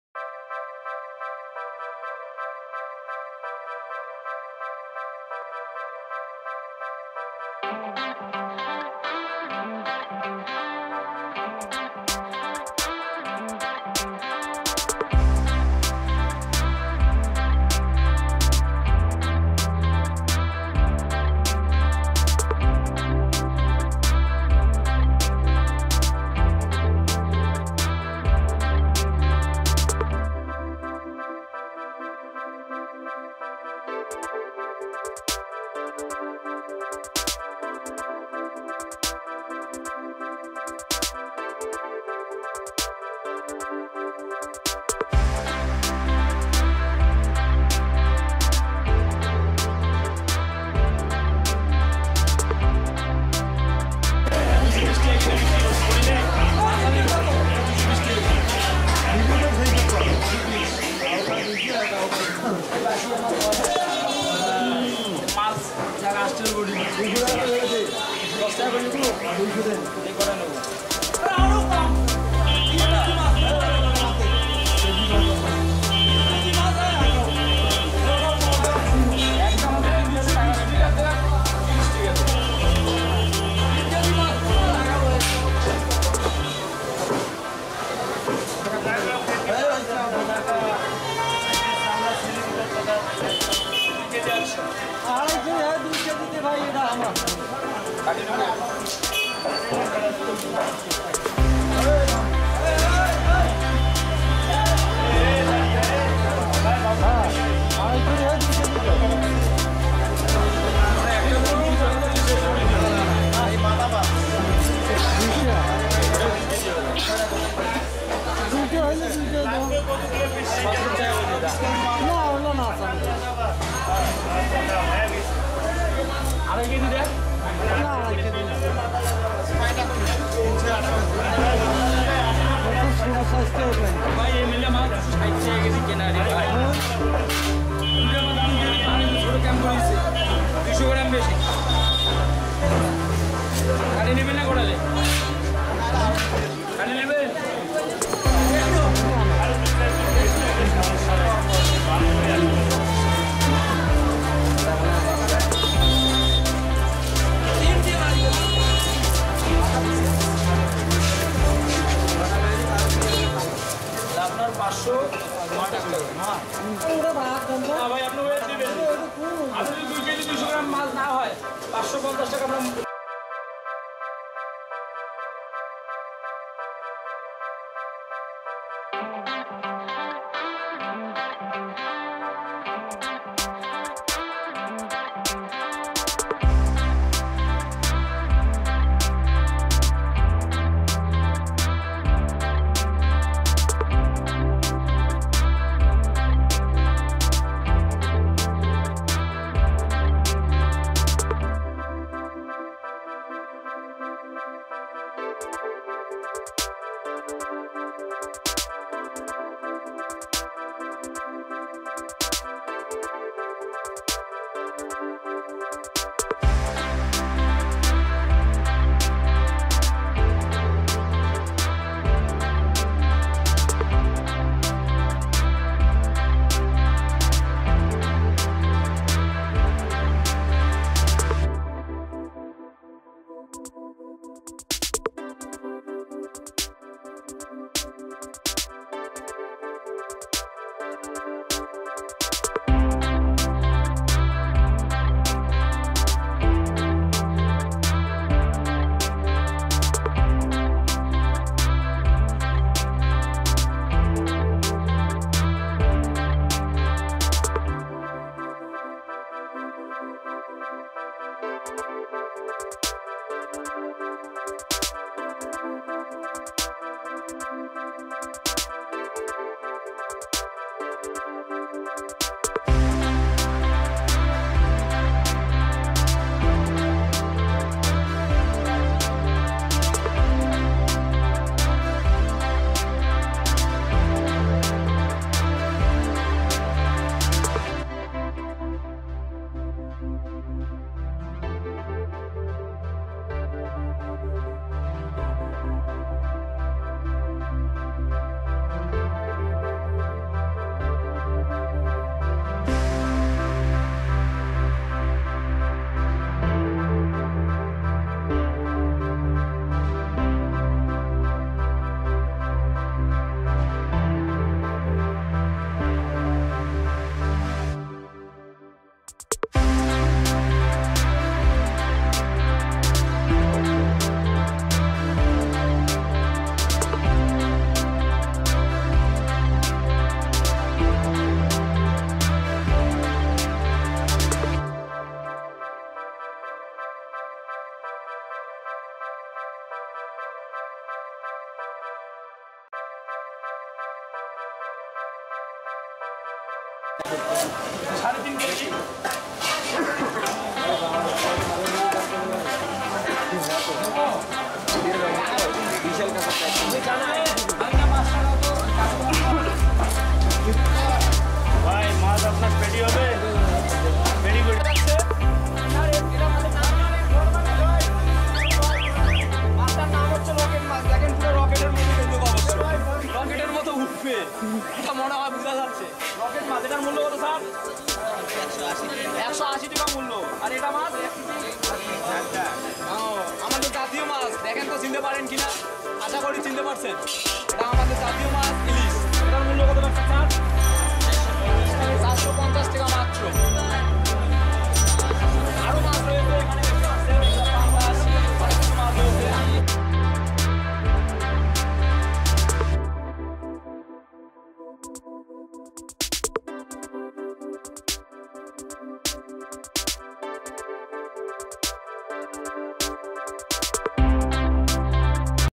Bangladeshi Amazing Village Fish Market & Fish Cutting Technique | Brothers Kitchen HD
Dear Viewers you are watching now Bangladeshi Amazing Village Fish Market & Fish Cutting Technique by Brorthers Kitchen HD

Stay with us and keep watching our videos in regular basis.